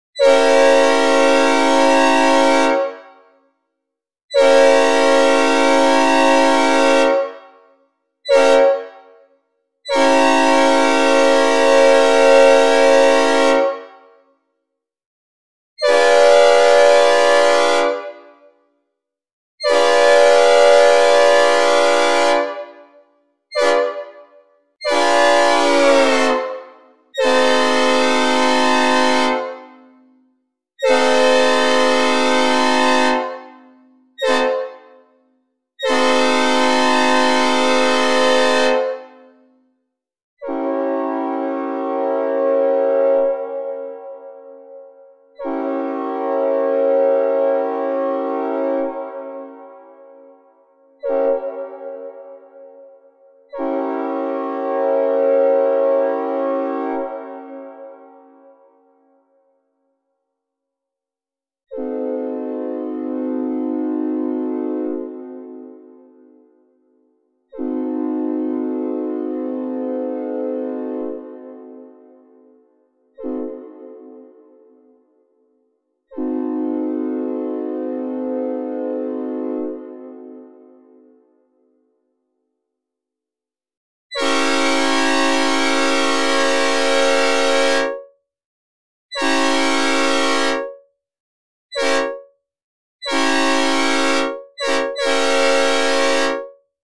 Nathan P5A “Old Cast” Train Horn (HQ Synthesizer Recreation)
i.e. The train horn that's banned in churches.

Recreated in FL Studio 20 using the “Wasp” synthesizer in combination with built-in effects such as Fruity Convolver, Reverb 2, and Flanger.